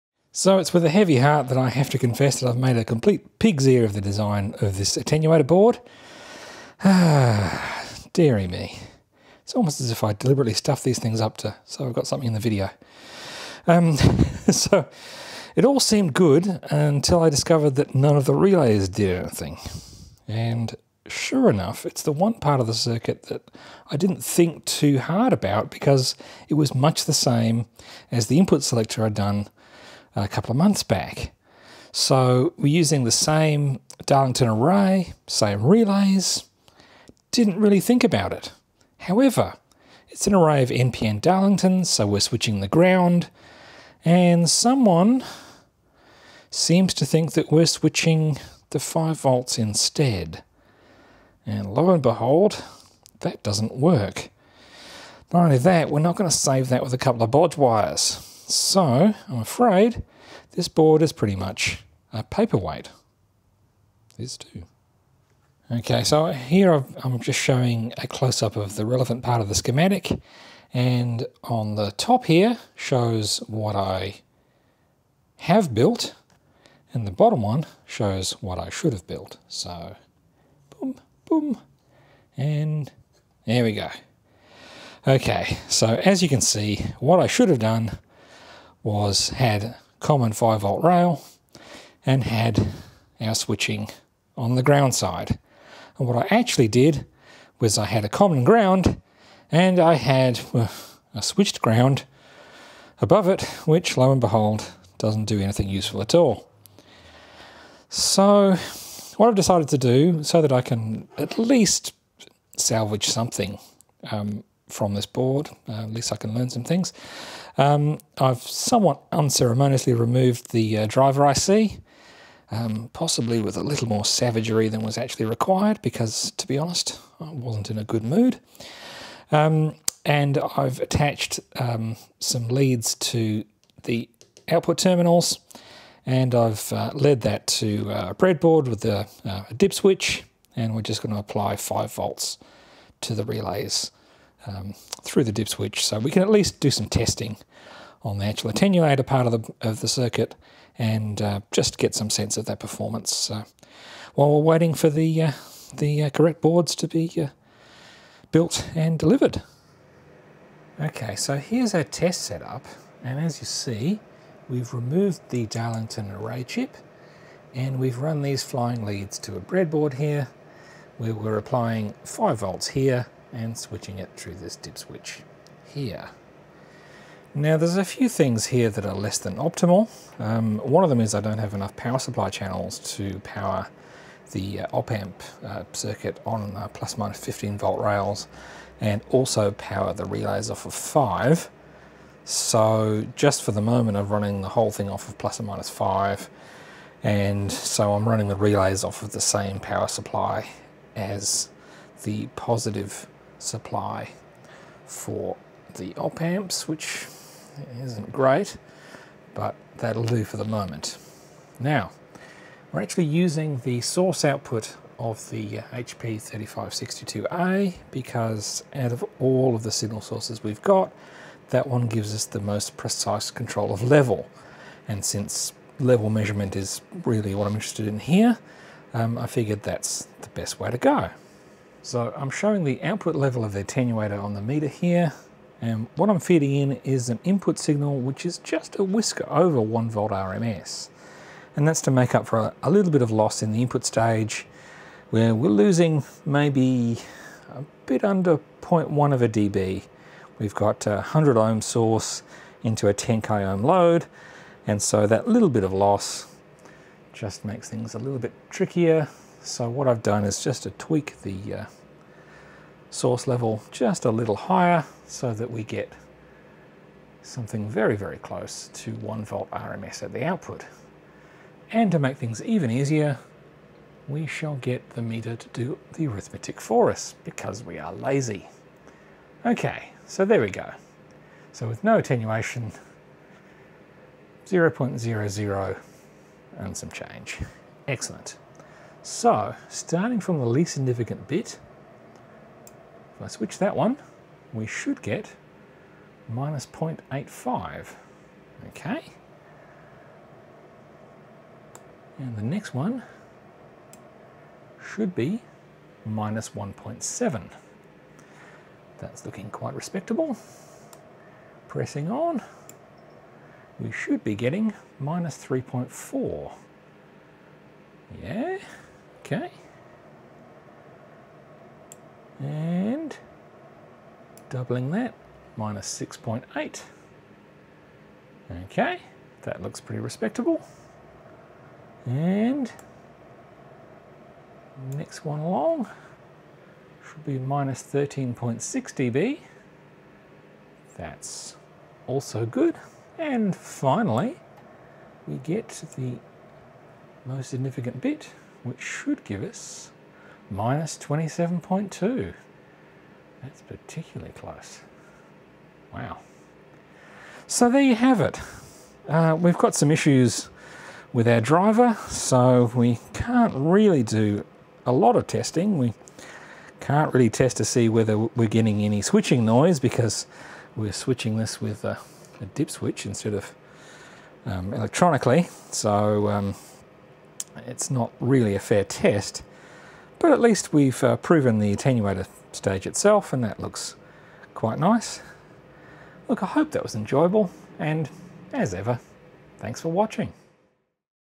DIY Amplifier Project - Attenuator PCB design error
I managed to stuff up the design of my attenuator circuit comprehensively enough to require a whole new PCB. Apart from that, it all went rather well...